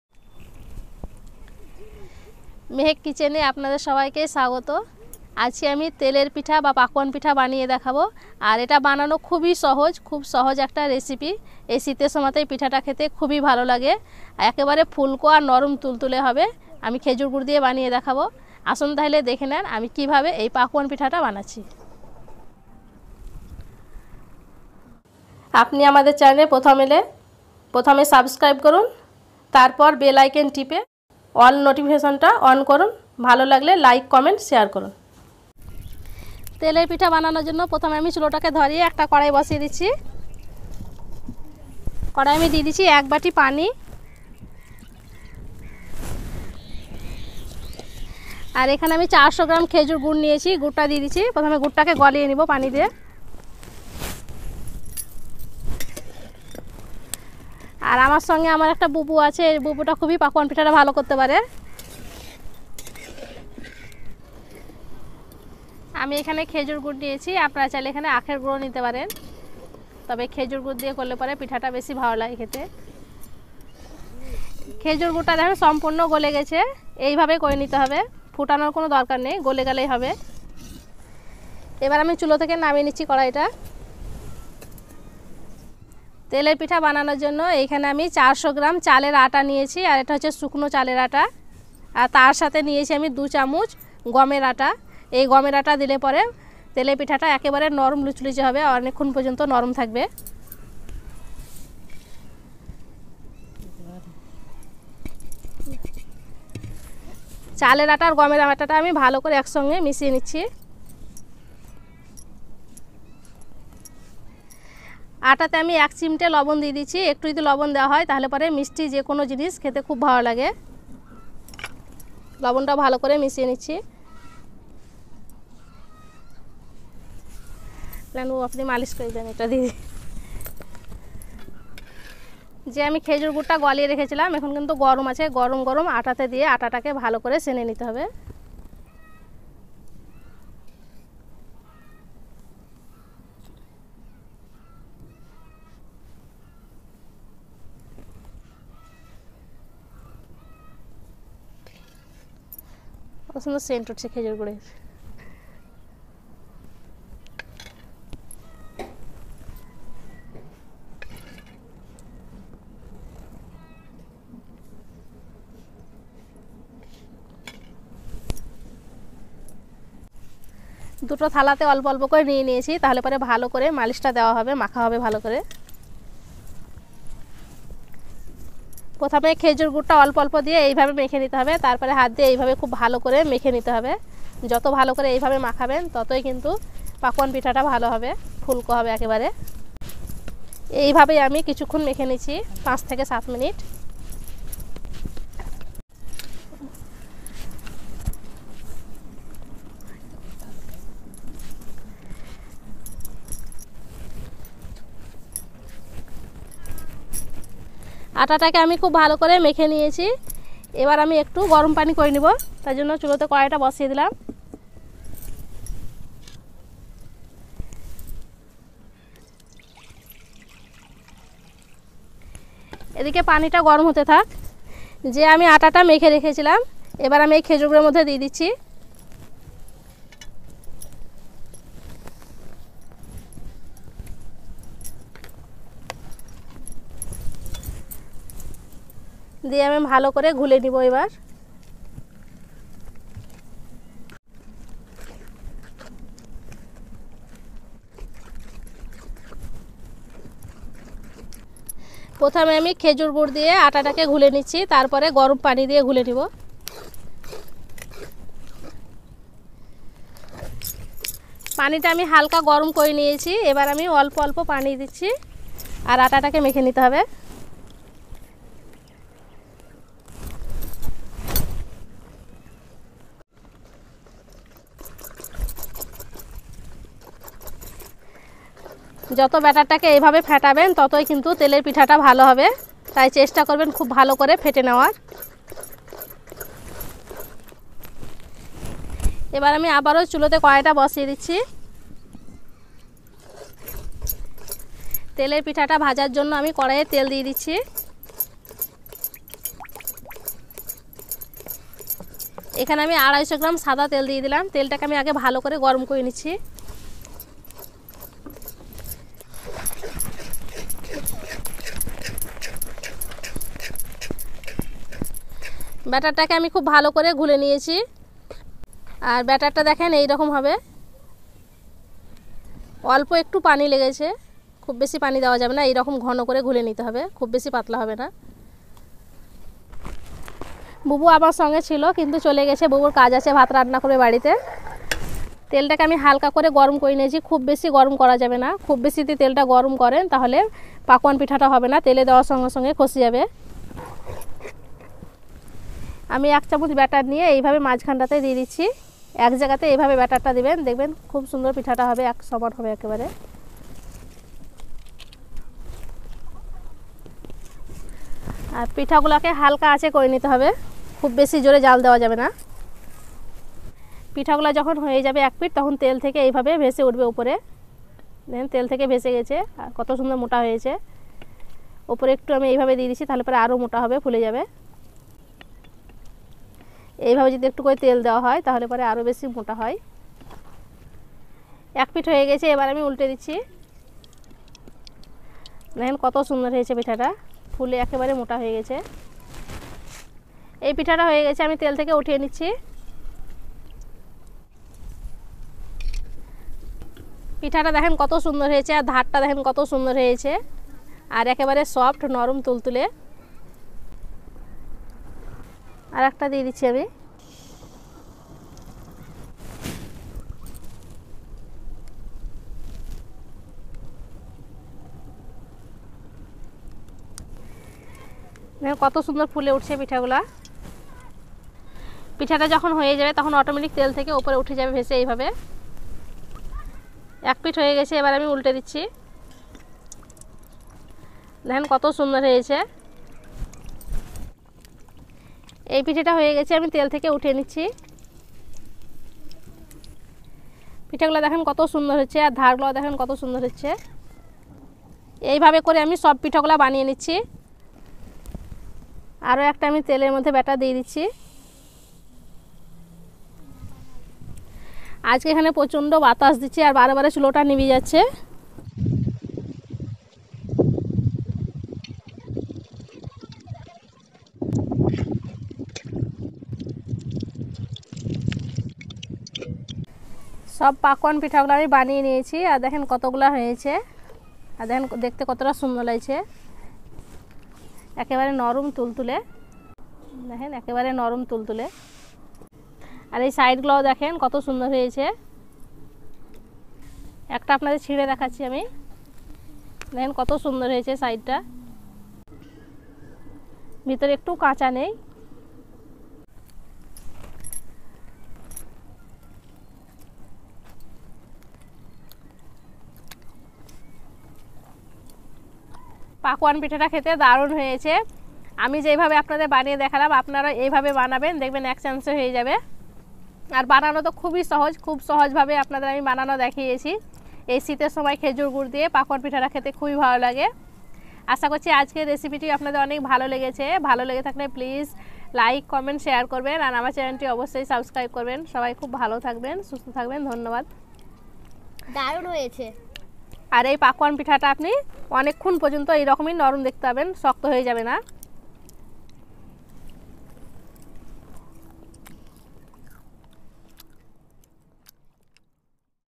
গ্রাম্য পদ্ধতিতে খুব সহজে পাকন পিঠা বা তেল পিঠা, যেটা 3 থেকে 4 দিন পর্যন্ত নরম তুলতুলে থাকবে
গ্রাম্য পদ্ধতিতে খুব সহজে পাকন পিঠা বা তেল পিঠা যেটা 3 থেকে 4 দিন পর্যন্ত নরম তুলতুলে স্পঞ্জি থাকবে
                    আজ আমি খুব সহজে পাকন পিঠা বা তেল পিঠা বানিয়েছি, আর খুব সহজভাবে বানিয়েছি, দেখলে পরে যে কেউ এটা বানিয়ে নিতে পারবেন